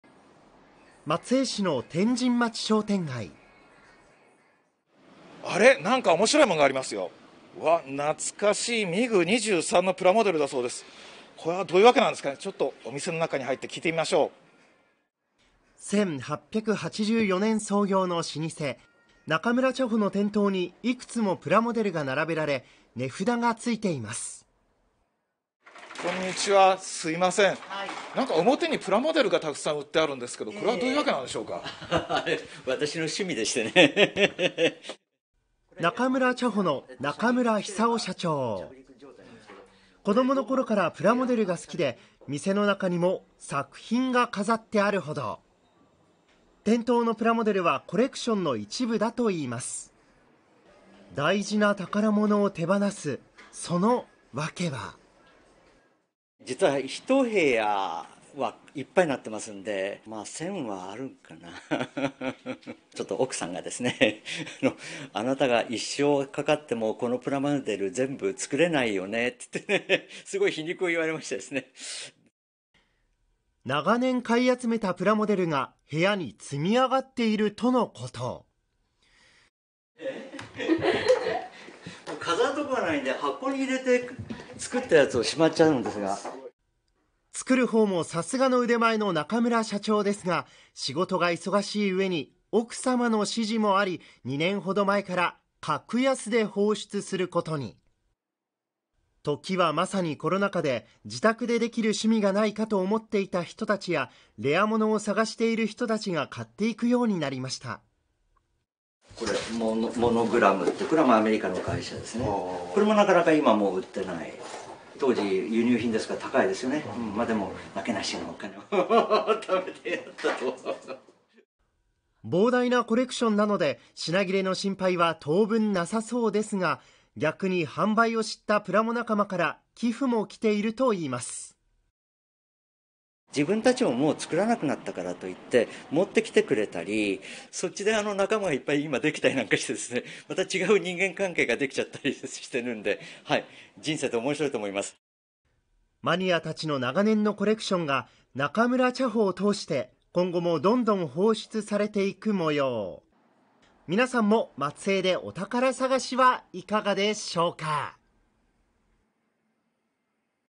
プレミアものも…秘蔵のプラモデルを茶店の店頭で格安販売！そのワケとは？
茶店の店頭にプラモデル。松江市の商店街で見つけたこの光景。しかも値段は格安で、プレミアものもあるかもしれないといいます。茶店でなぜプラモデルを販売するのか？社長を直撃しました。